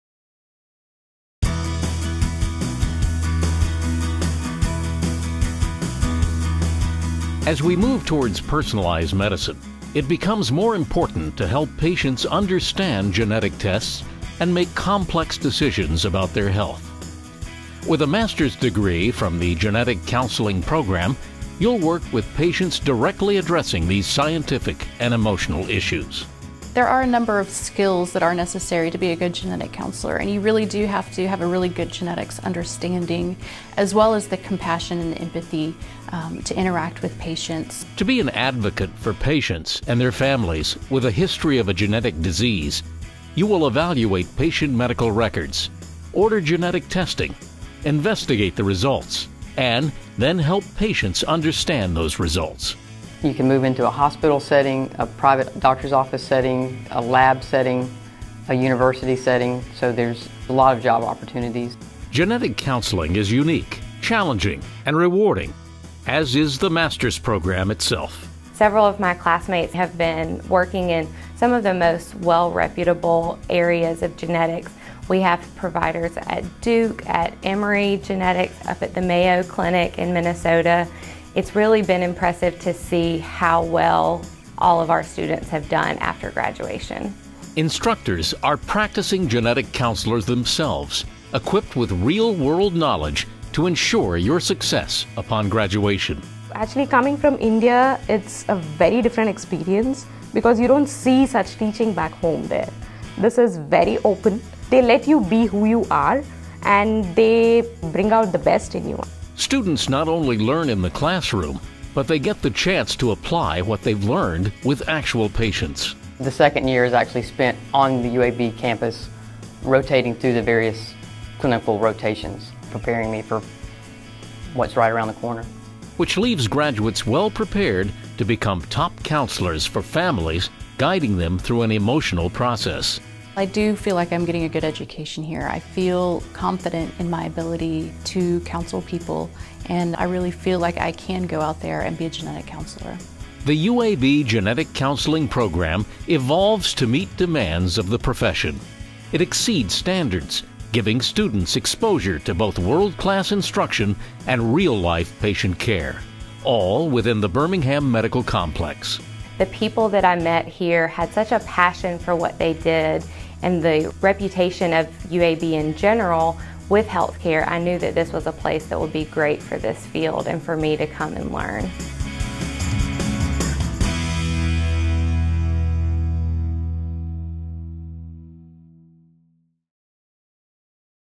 UAB Genetic Counseling
The University of Alabama at Birmingham's Master of Science in Genetic Counseling program will prepare you to work with patients and families who are learning about and coping with the risk genetic disease may have for themselves and their relatives. These include medical and emotional needs that affect their healthcare and their daily lives.

You will learn best practices for genetic risk assessment, education, and counseling. You will be able to promote informed choices and adaptation to a genetic risk or genetic diagnosis.

The UAB Genetic Counseling program combines scientific and clinical education to prepare you for the evolving field of genetic counseling and to work with a variety of patients.